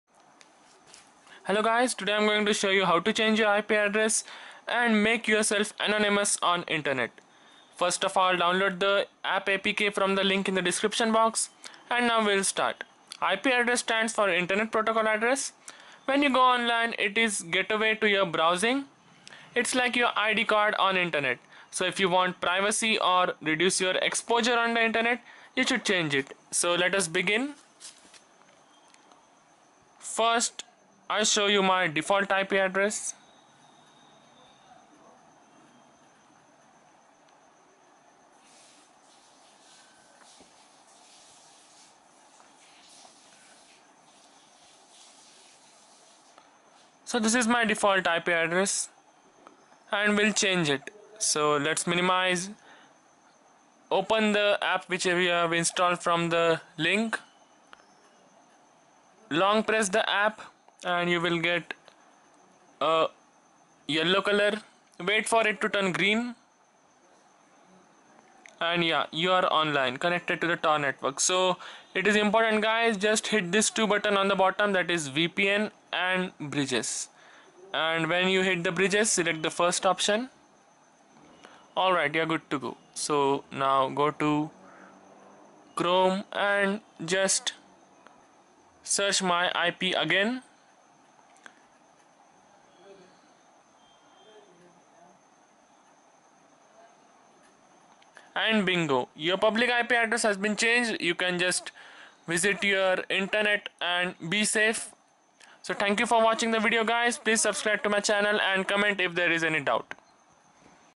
How to Change IP Address of Android Phone
Stay Away from Hackers , Stay Away for Spying.Change you IP Address and Browse Freely on Internet

Follow the video and get your IP address changed